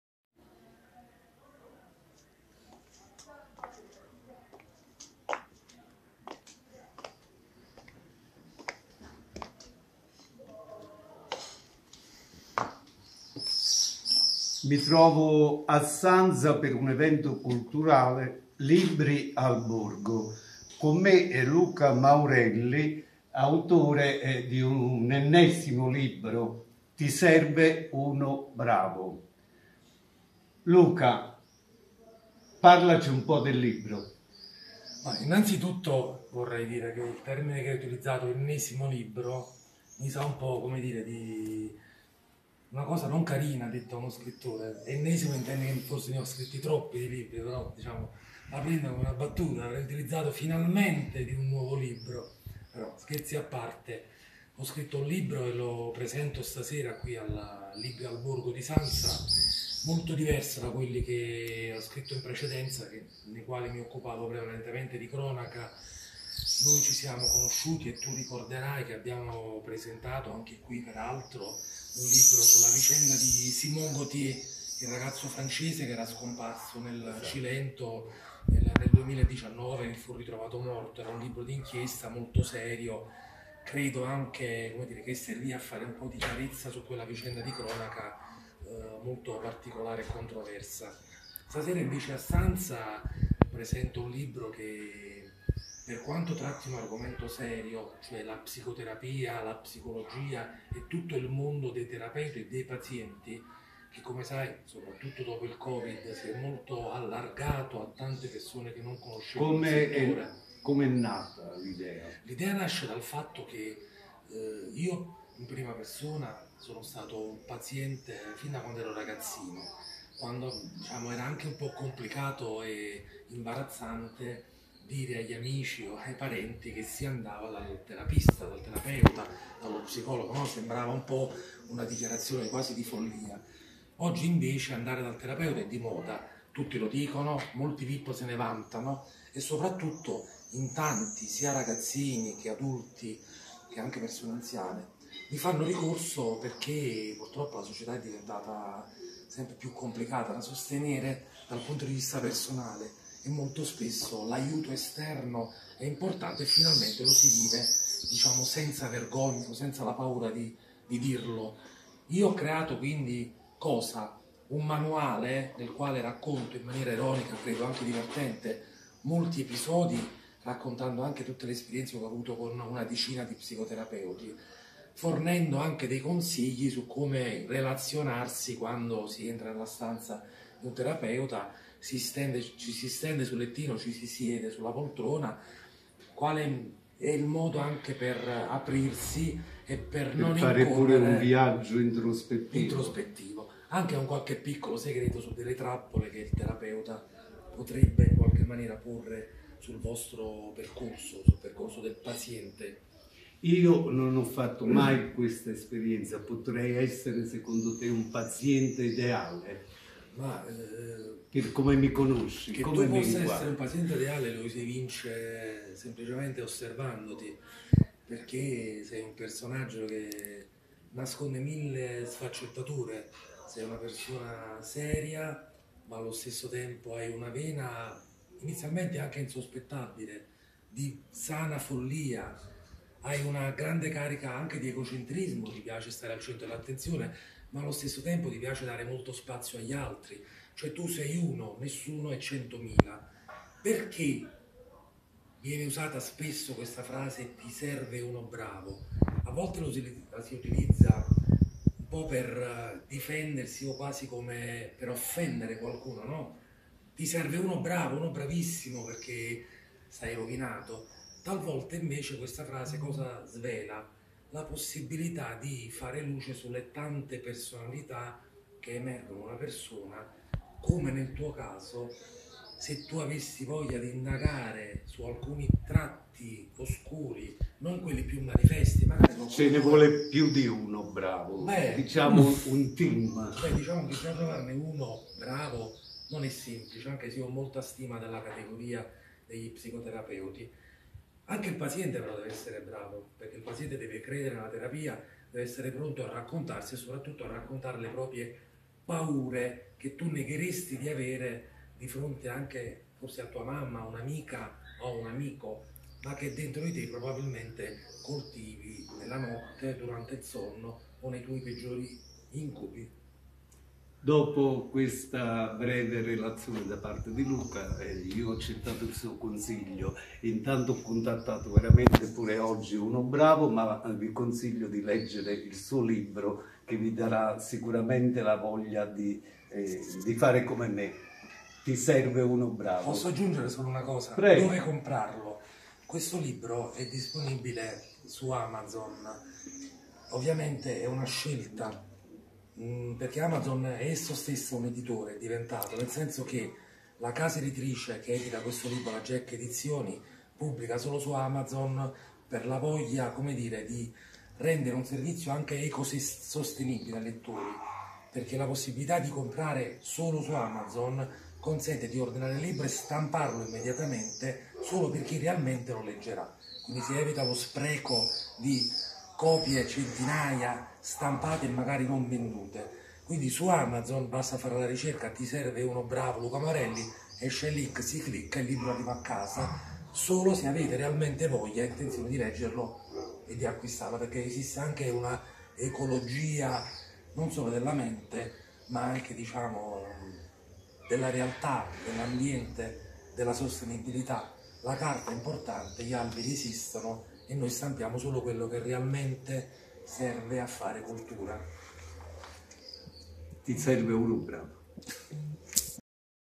Sanza, Evento
Sanza, Evento LibrialBorgo2023 ideato e condotto da Lorenzo Peluso .
In questa prima serata Luca Maurelli ha presentato il suo ultimo libro dal titolo "Ti serve uno bravo"